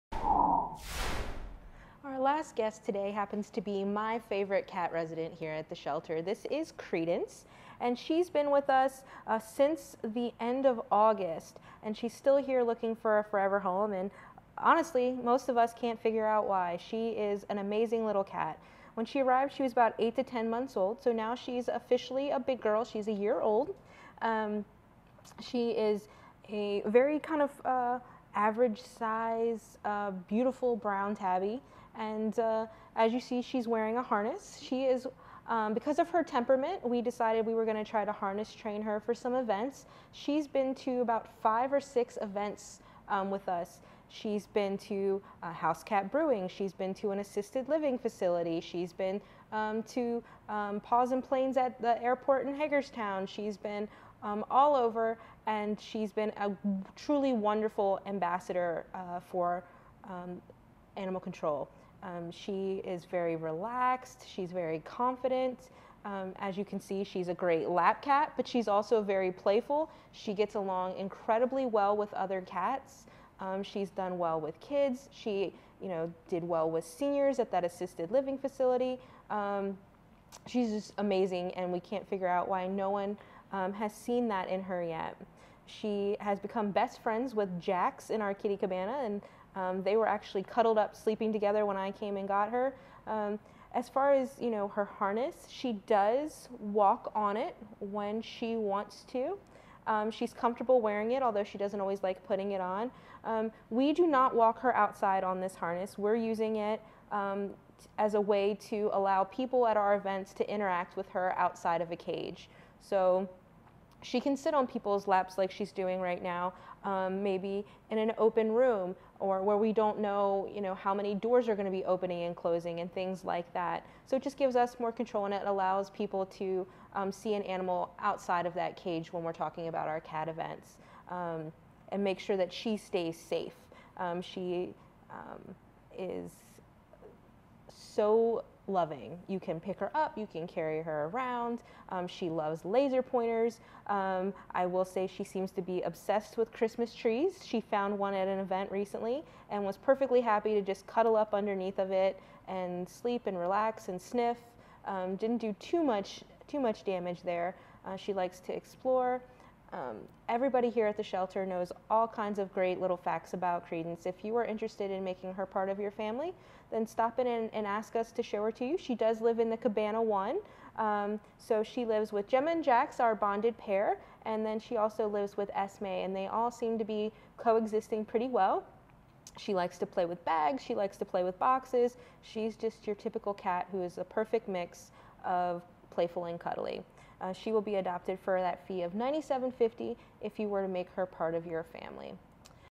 Credence - 190703
Meet Credence, a Domestic Shorthair / Mix, Age: 1y, Gender: Female, Color: Brown / White

Color Code: Orange